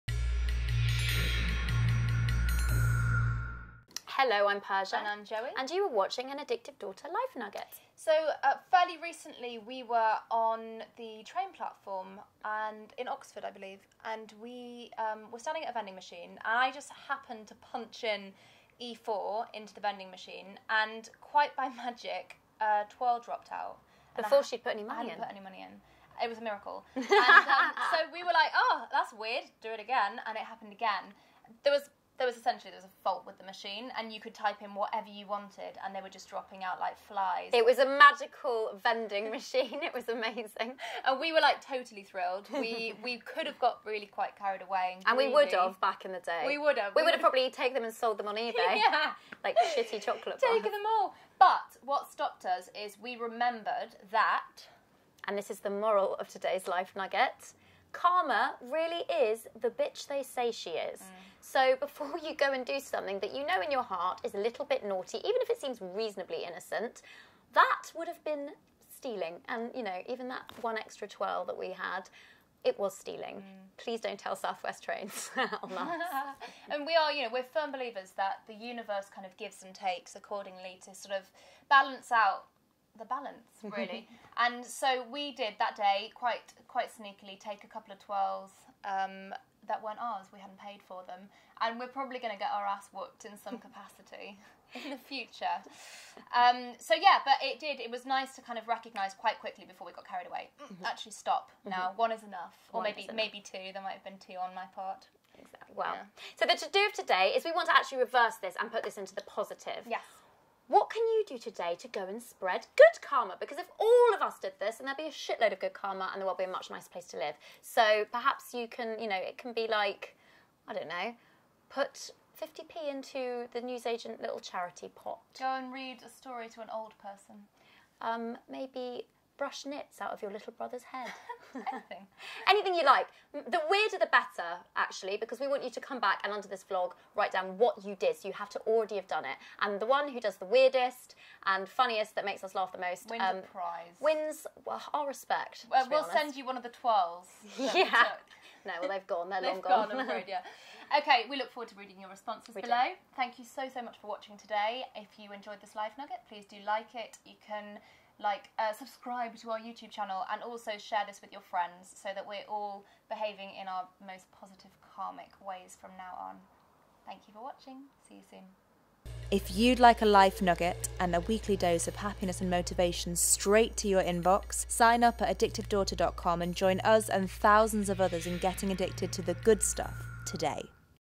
How to Attract the Good & Repel the Bad
Have you ever been tempted to do something in the spur of the moment that you just knew deep down was probably going to have negative consequences somewhere down the line?

For example, finding a brand new iPhone on the floor of a shop and choosing to sell it on Ebay, rather than handing it in.

Or, lying to your friend that you’re ill and can’t go to her party, when in fact you’re on your way to meet the tall dark handsome stranger that asked you on a date last night.

There are countless ways we can trick ourselves into justifying our not-so-loving behavior, but in the long run, consciously making bad choices always catches up with us.

Always.

In today’s Life Nugget, we’ll share with you the number one reason you SHOULDN’T make the same mistake we did recently, as well as our top tip to help you attract only GOOD stuff into your life.

After watching the video, let us know what weird & wonderful act you performed today to spread the love & attract karma of the happy kind.

We can’t wait to read your comments!

Big love,
Persia & Joey
xx